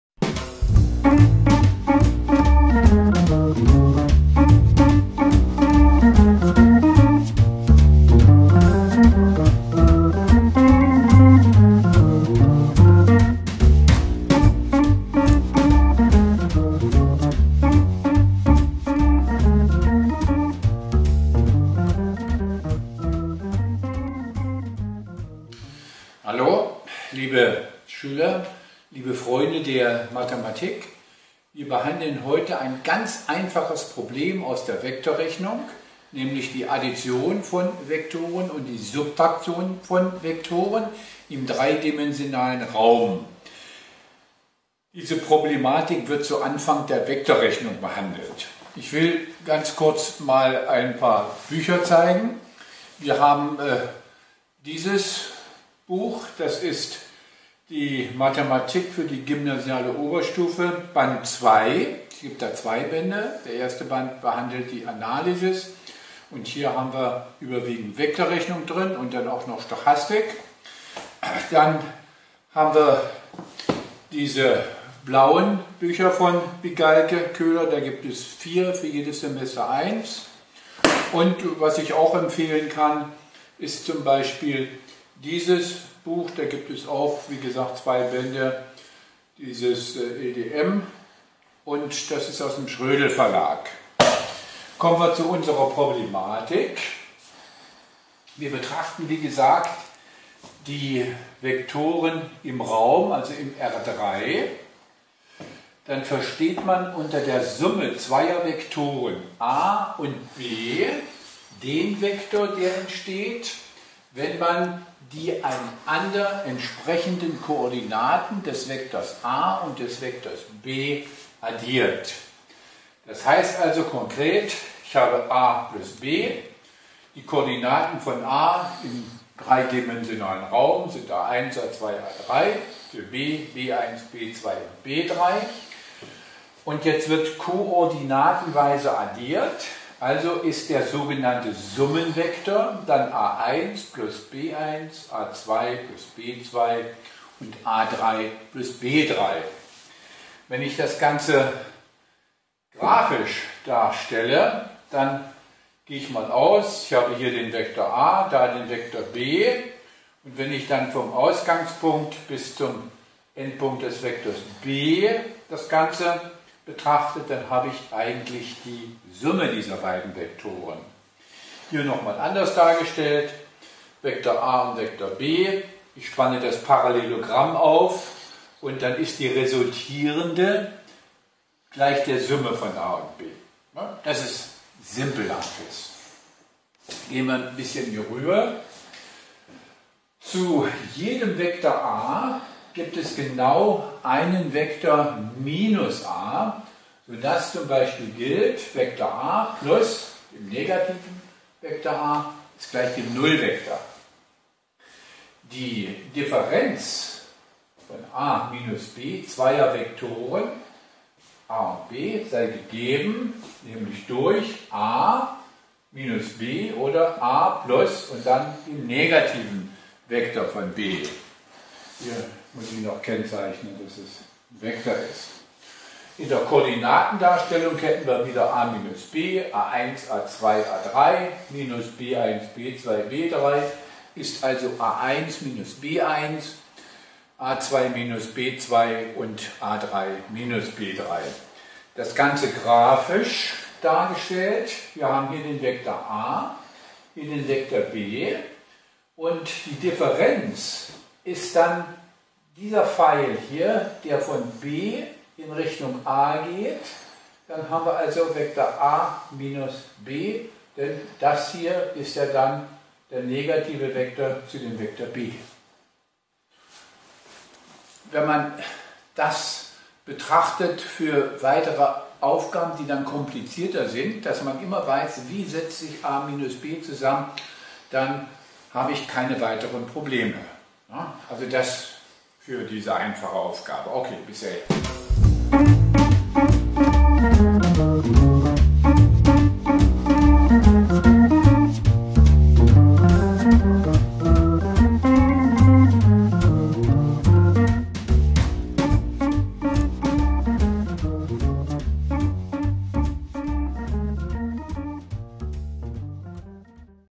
Addition und Subtraktion von Vektoren
Es wird die addition und Subtraktion von Vektoeren grafisch und analytisch gezeigt.